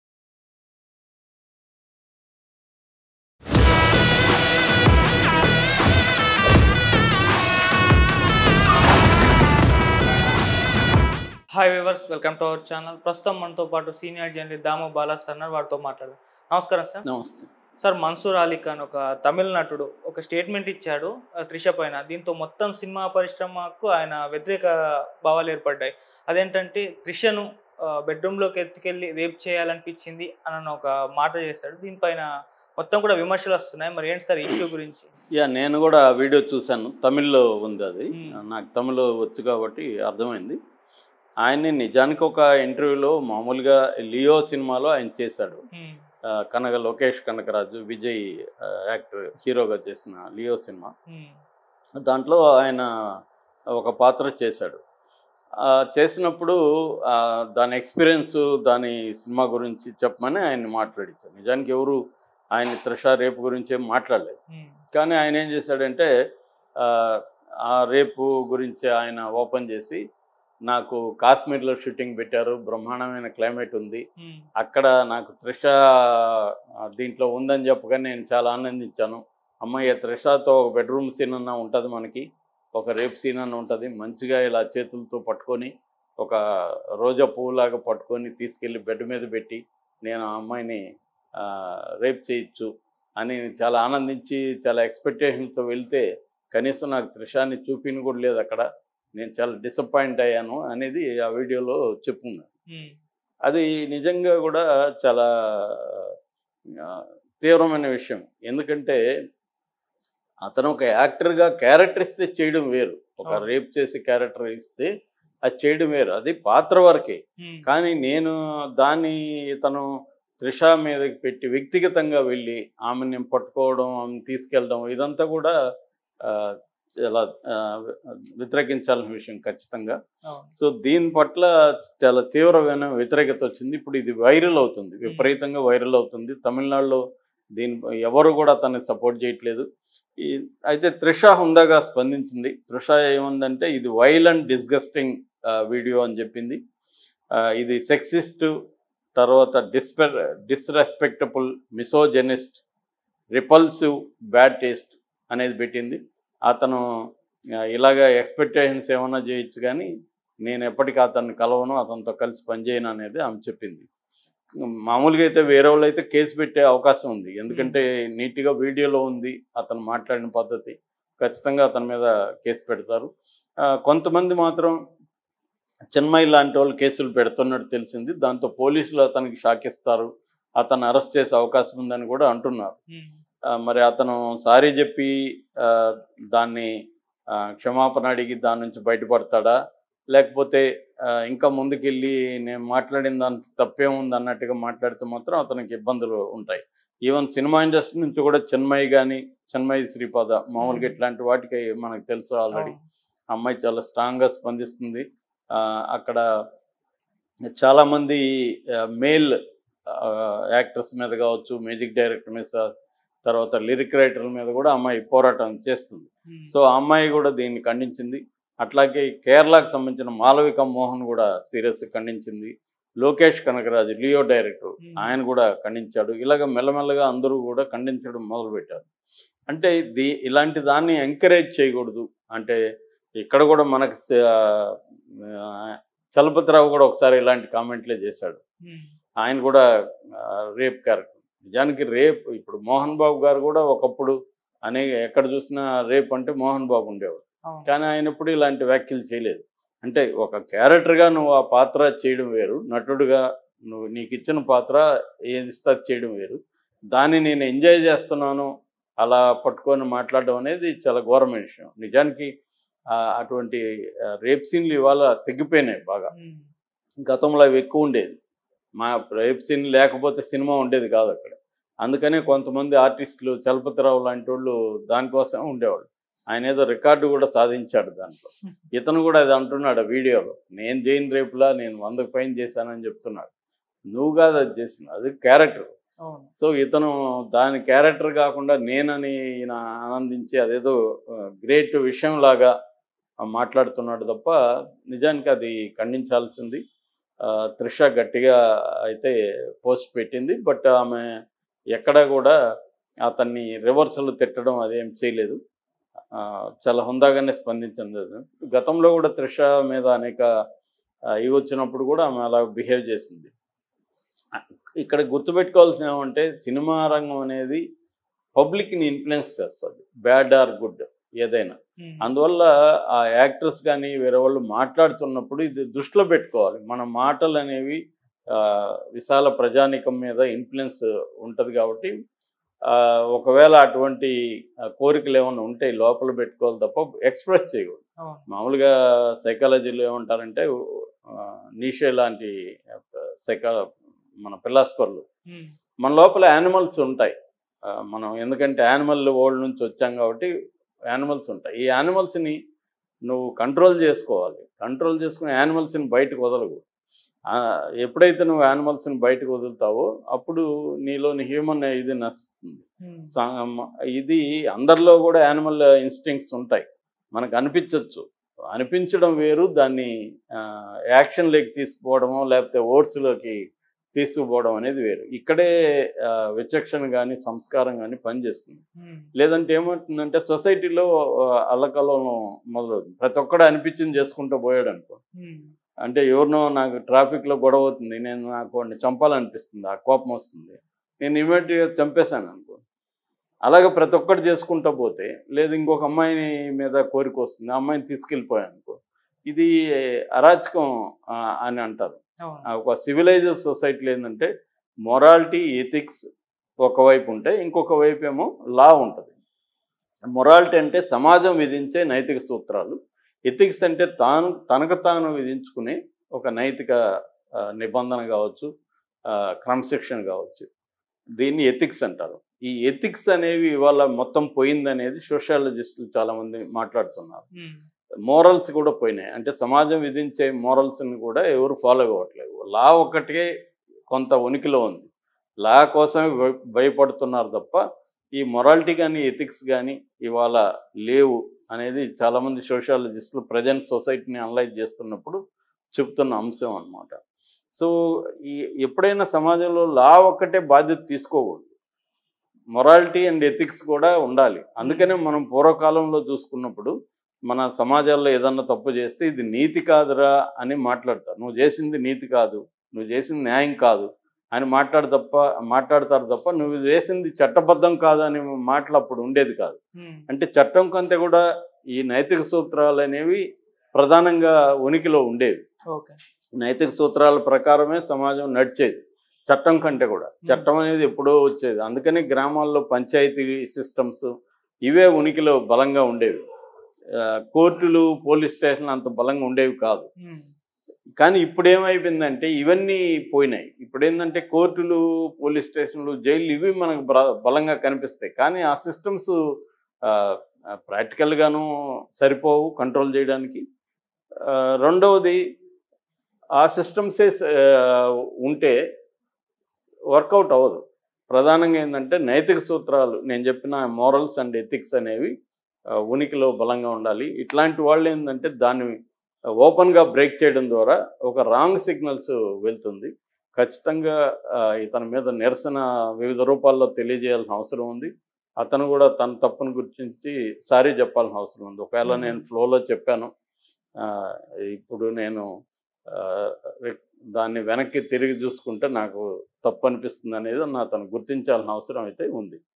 త్రిషను రేపు చేయాలనిపించింది నటుడు మన్సూర్ అలీ ఖాన్ సంచలనం | Trisha Vs Mansoor Ali Khan Controversy
త్రిషను రేపు చేయాలనిపించింది నటుడు మన్సూర్ అలీ ఖాన్ సంచలనం | Trisha Vs Mansoor Ali Khan Controversy | Daamu Balaji Diaries